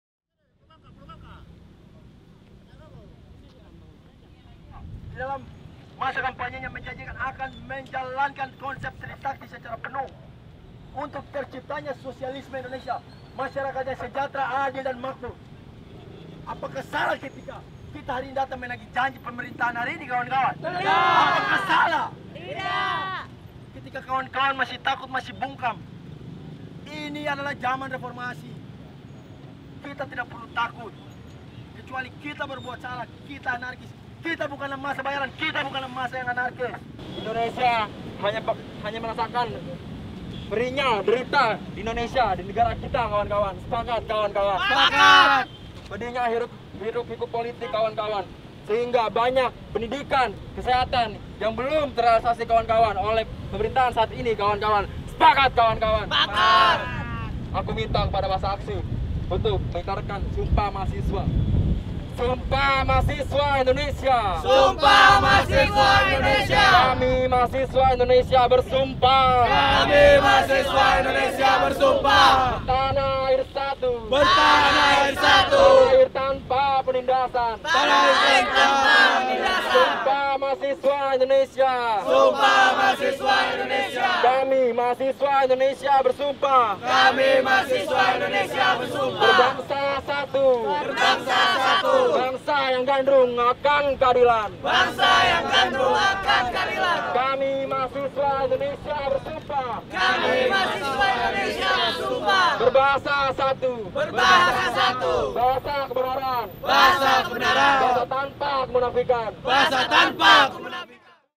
Sorgemagz.com - Aksi Famber (Forum Aksi Mahasiswa Bersatu) l 29 Mei 2015
Puluhan mahasiswa yang tergabung dalam Famber (Forum Aksi Mahasiswa Bersatu) menggelar aksi di depan Istana Negara, Jakarta, Jumat 29 Mei 2015. Mereka menuntut Jokowi-JK untuk merealisasikan janji-janji di masa kampanye, karena menurut mereka saat ini Indonesia tunduk dalam cengkraman kaum neoliberalisme. Adapun mahasiswa yang tergabung dalam Famber adalah mahasiswa dari HAMAS Universitas Nasional, Universitas Bung Karno, dan kampus lainnya. Simak sekilas video aksi tersebut.